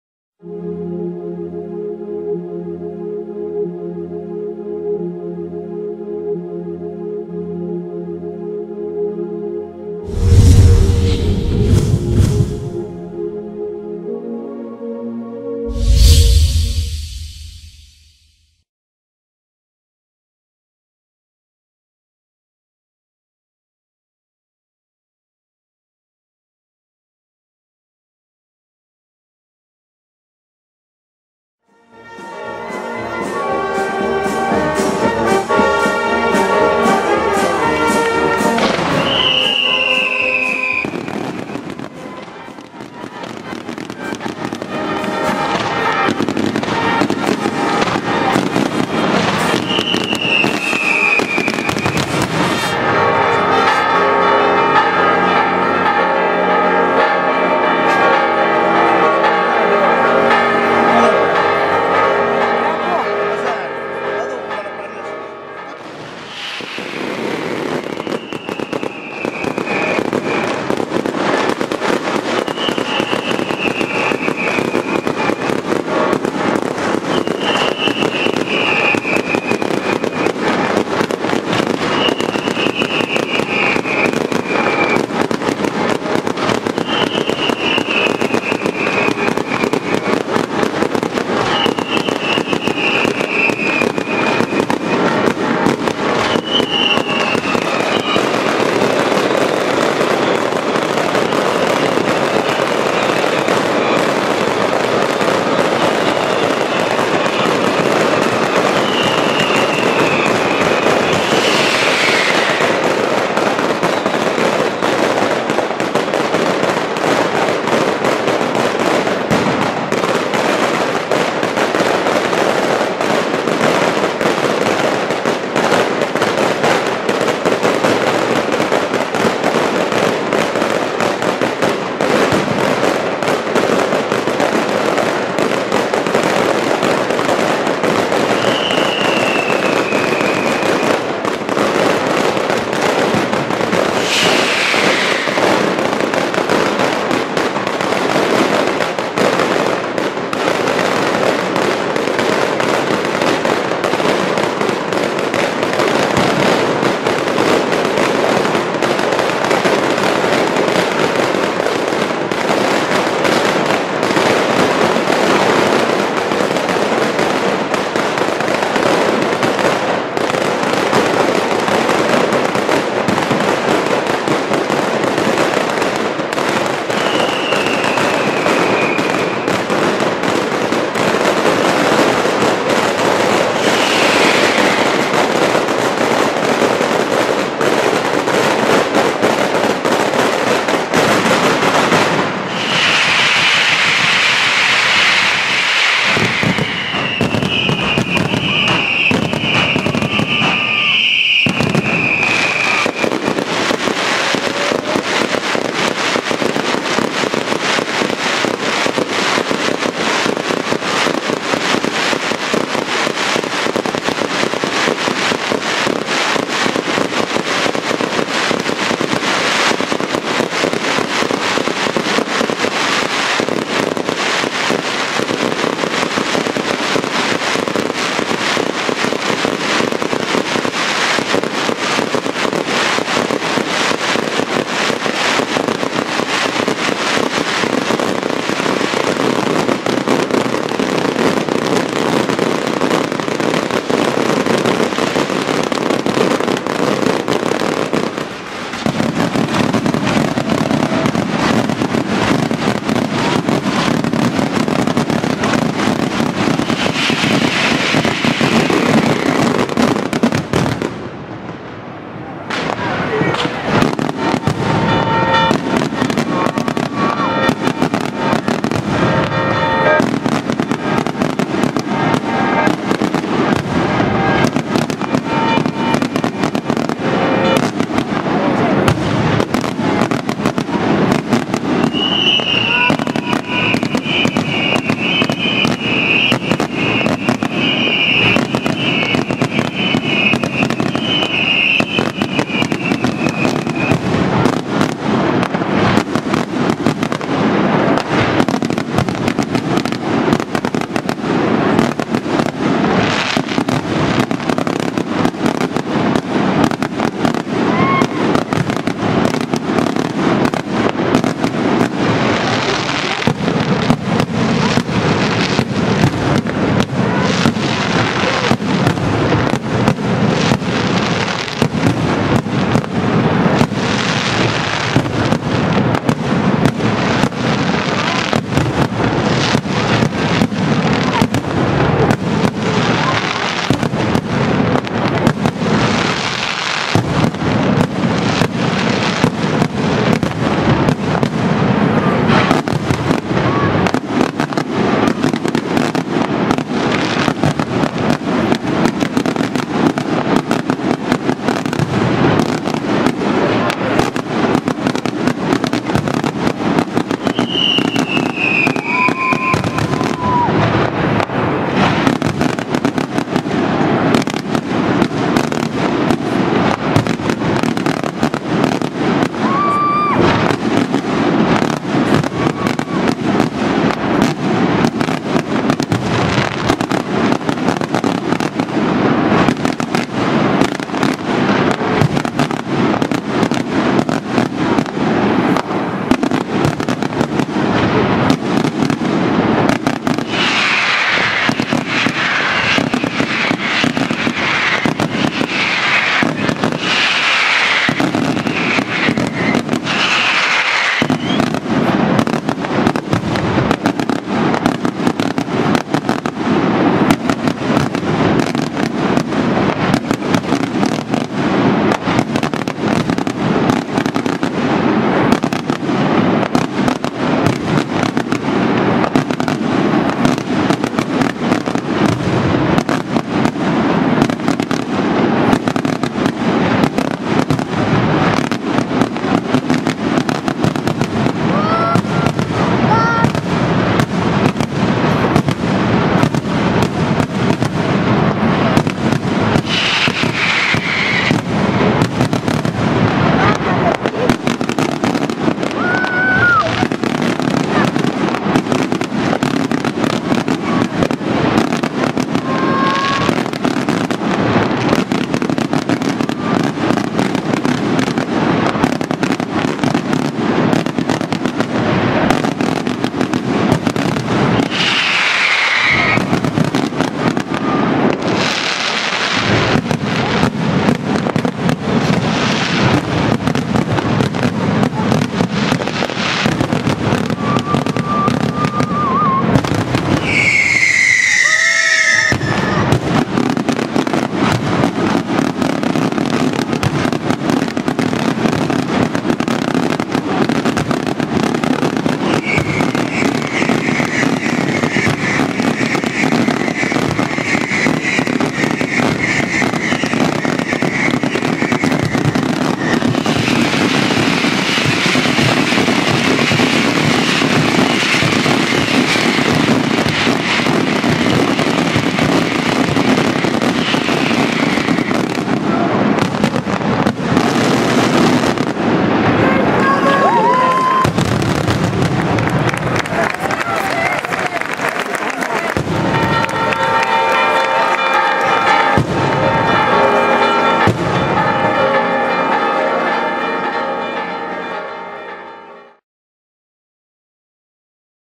BELPASSO (Ct) - Santa LUCIA V.M. 2019 - FUOCHI CHIARENZA (Daylight Show)
Belpasso (Ct) - 13 Dicembre 2019
Festività di Santa Lucia V.M.

Trionfale Uscita - Spettacolo Pirotecnico Diurno
FUOCHI CHIARENZA da Belpasso (Ct)
Riprese video Simone Vinciguerra